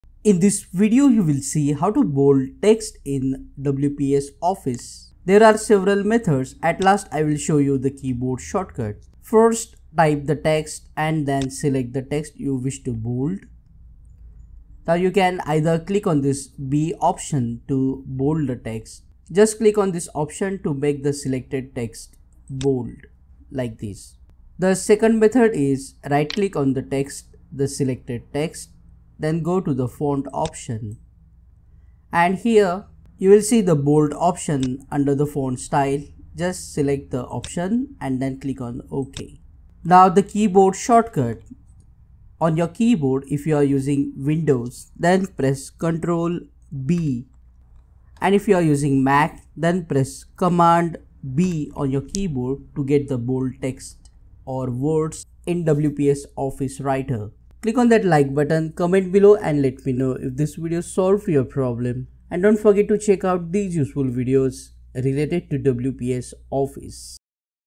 How To Bold Text In WPS Office
Watch in this video How To Bold Text In WPS Office Writer. Make use of Bold text option under the home tab or use the Font option to bold the text, words, or paragraph sentences. You can also use the Keyboard shortcut CTRL + B (Windows ) and CMD + B (MAC) to get the Bold text in WPS Office